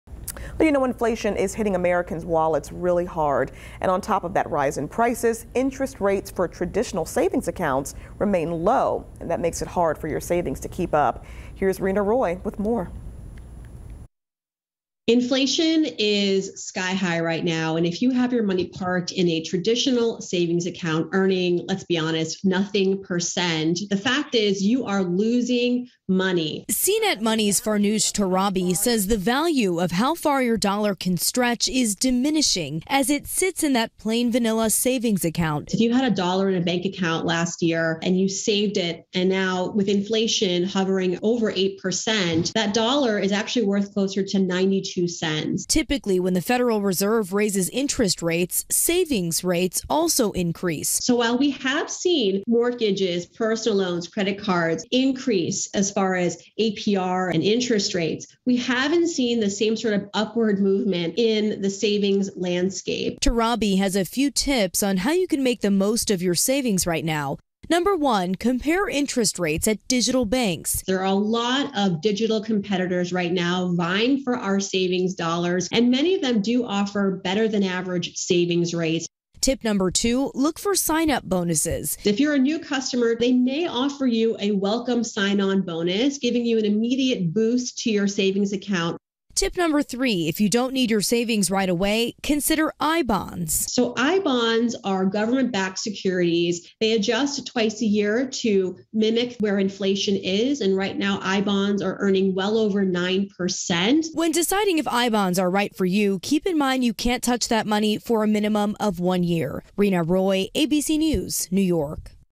Maximizing your savings amid inflation
Inflation is hitting Americans' wallets hard and on top of the rise in prices, interest rates for traditional savings accounts remain low, making it hard for your savings to keep up.  Here's ABC's Reena Roy with more.